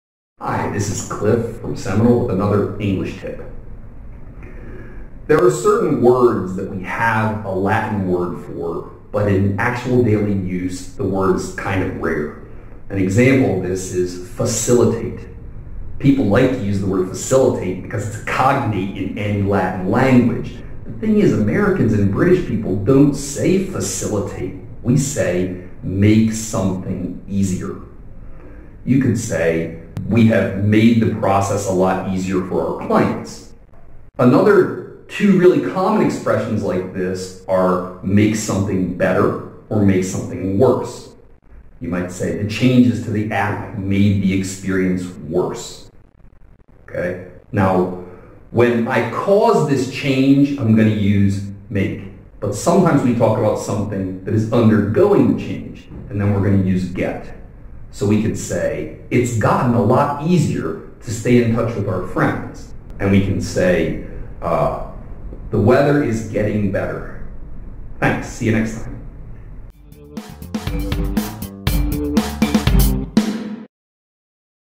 CONVERSATION: Make (something) easier
Há várias palavras em inglês de origem latina, mas que não são usadas com muita frequência.

Um exemplo é "facilitate". As pessoas gostam de usar esta palavra porque ela é um cognato do latim, mas os nativos da língua inglesa como americanos e britânicos não a utilizam em sua linguagem cotidiana.

Nativos geralmente dizem: "make something easier" para dizer "facilitar" em português.

Podemos dizer: "We've made the process a lot easier for our clients".

Similar a esta expressão temos: "make something better" ou "make something worse".

Como por exemplo:

"The changes to the app made the experience worse".  As mudanças no aplicativo tornaram a experiência pior.

Pode também usar "get" quando não há uma pessoa/coisa responsável pela melhora ou piora da situação.

"It's gotten a lot easier to stay in touch with our friends". Ficou muito mais fácil entrar em contato com nossos amigos.

"The weather is getting better". O tempo está ficando melhor.